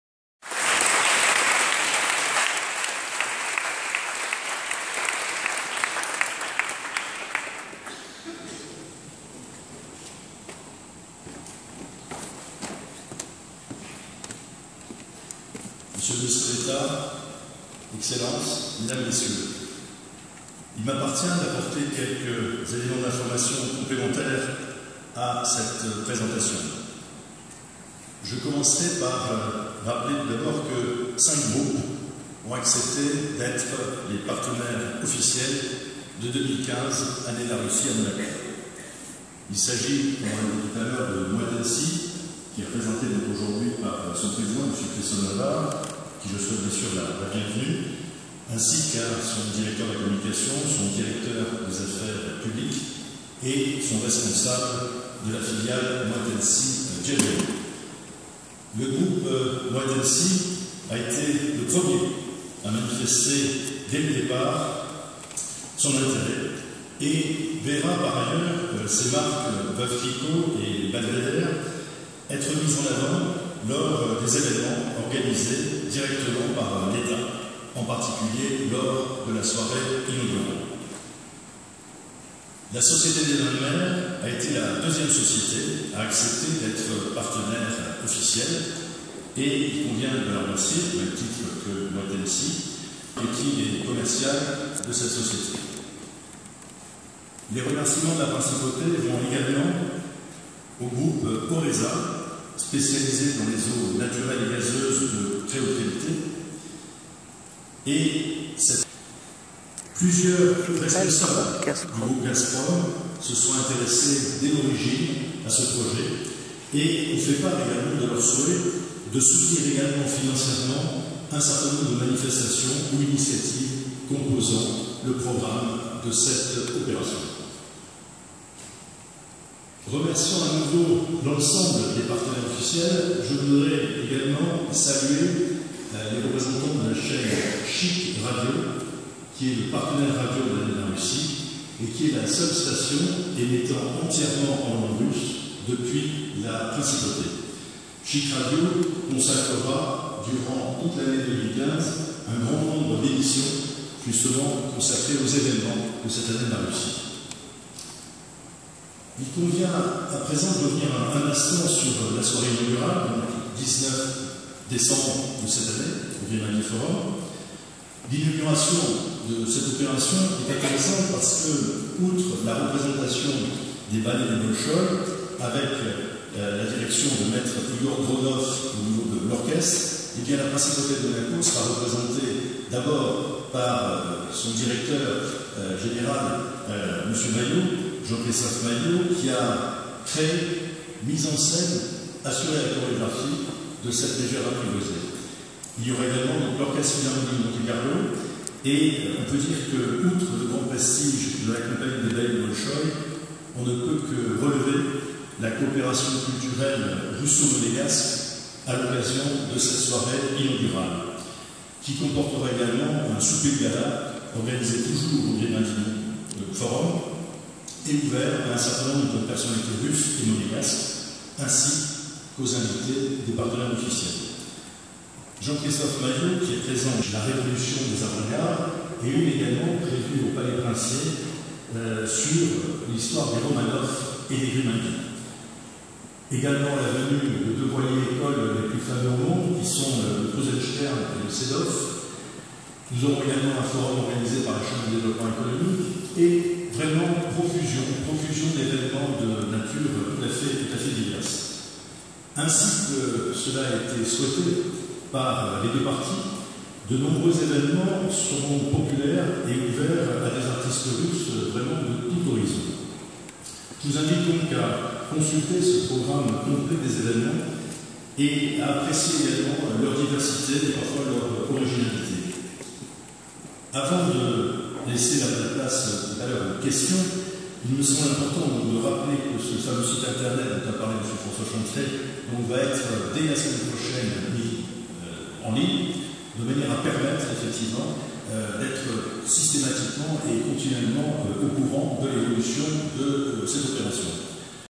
S.E. M. Henri Fissore/ 2015 année de la Russie à Monaco. Conférence de presse .
S.E. M. Henri Fissore, Ambassadeur en Mission auprès du Ministre d'Etat. 2015 année de la Russie à Monaco. Inauguration le 19 Décembre avec les ballet du Bolchoi dans une mise en scène et une chorégraphie de Jean Christophe Maillot pour  "La mégère apprivoisée". Nombreuses manifestation au programme